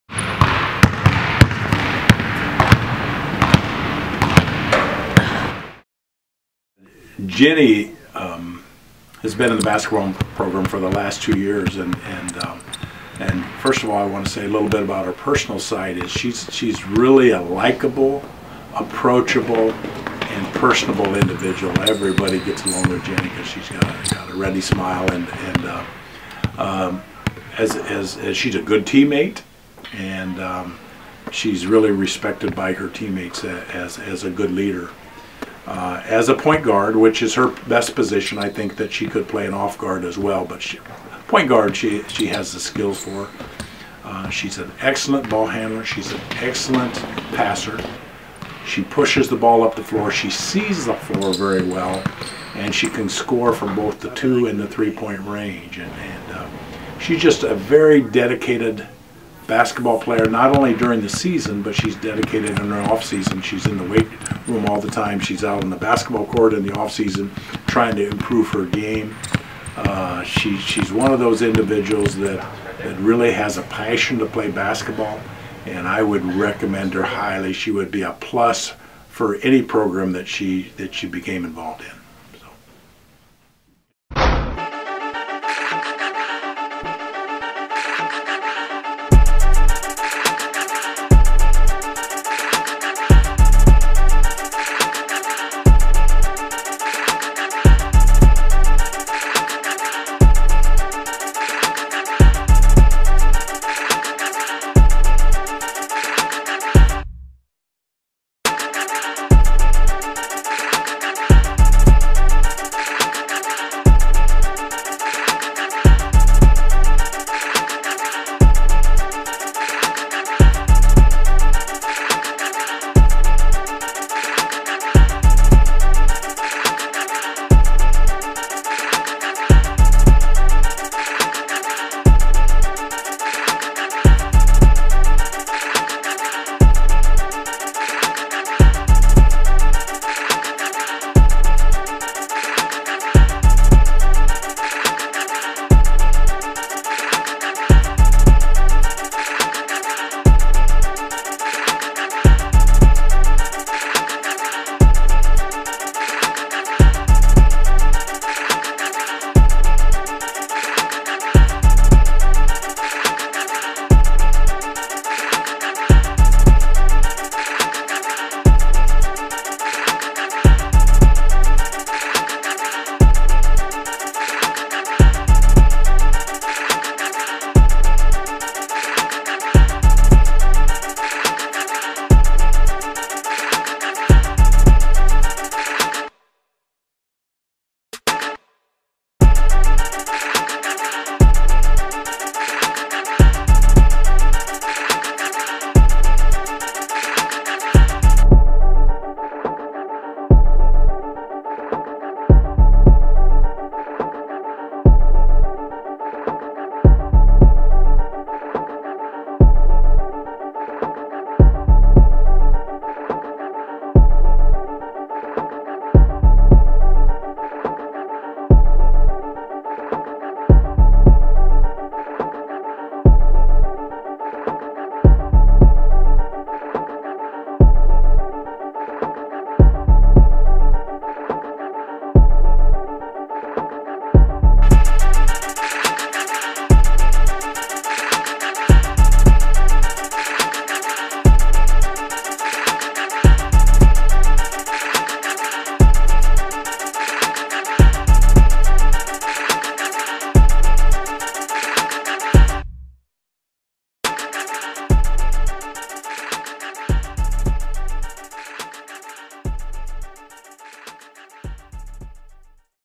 Ginny Sanchez Promo/Highlights
Highlights from pro prospect Ginny Sanchez (#10) at Waldorf College in Forest City, Iowa. Sanchez is looking to play professionally and is willing to play anywhere; US and overseas.

Filmed/Directed by Chris Oliver
Edited by Preston Landon